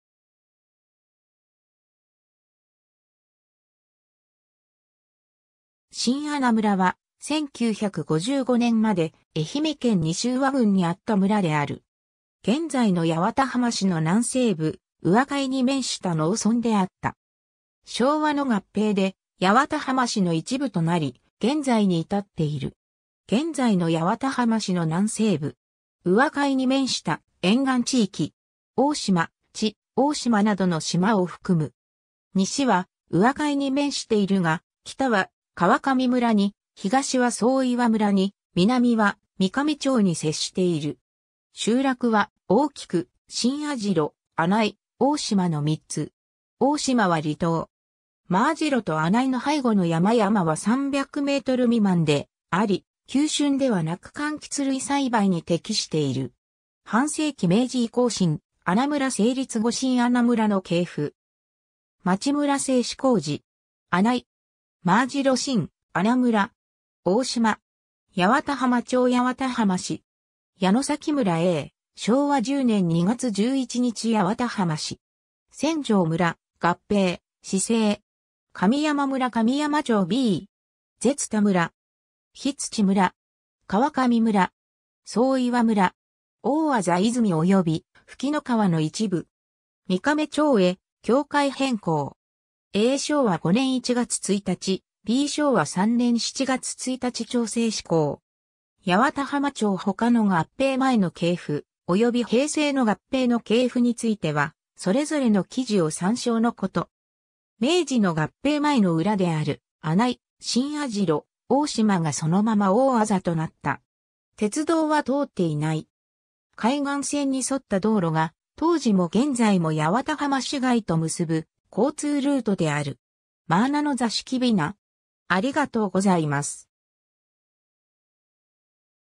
真穴村
真穴村（まあなむら）は、1955年（昭和30年）まで愛媛県西宇和郡にあった村である。
 現在の八幡浜市の南西部、宇和海に面した農村であった。
昭和の合併で八幡浜市の一部となり、現在に至っている。
 現在の八幡浜市の南西部。
宇和海に面した沿岸地域。
大島、地大島などの島を含む。
西は宇和海に面しているが、北は川上村に、東は双岩村に、南は三瓶町（一足先に1月1日に合併により成立）に接している。
集落は、大きく、真網代、穴井、大島の3つ。
大島は離島。
真網代と穴井の背後の山々は300m未満であり、急峻ではなくかんきつ類栽培に適している。
 藩政期 明治以降 真穴村成立後 真穴村の系譜
（町村制実施以前の村）　（明治期）
　　　　　　　　　　　　町村制施行時
穴井　　　━━━┓
真網代　　━━━╋━━　真穴村　 ┓
大島　　　━━━┛　　　　　　　　　　　　　　　　　　　　　　　　　　　　　　┃
八幡浜町　　━━┳ ┳八幡浜市　 ┃
　　　　　　　　　　　　矢野崎村　　━━┛ａ      ┃（昭和10年2月11日　　　　┣━八幡浜市
千丈村　　 ┫　、合併、市制）　　　　　┃（昭和30年2月1日）
　　　　　　　　　　　　神山村    ━━━神山町ｂ━┫　　　　　　　　　　　　　┃
舌田村 ┛　　　　　　　　　　　　　┃
日土村 ┫
川上村 ┫
双岩村 ┳┛
　　　　　　　　　　　　　　　　　　　　　　　　　　　　　　　　　大字和泉及び布喜川の一部
　　　　　　　　　　　　　　　　　　　　　　　　　　　　　　　　　三瓶町へ（昭和30年1月1日）境界変更

a 昭和5年1月1日、b 昭和3年7月1日町制施行
（注記）八幡浜町ほかの合併前の系譜、及び平成の合併の系譜については、それぞれの記事を参照のこと。

 明治の合併前の浦である、穴井（あない）、真網代（まあじろ）、大島（大島）がそのまま大字となった。
 鉄道は通っていない。
 海岸線に沿った道路が当時も現在も八幡浜市街と結ぶ交通ルートである。
（愛媛県道28号を経て、現在の国道378号） 真穴の座敷雛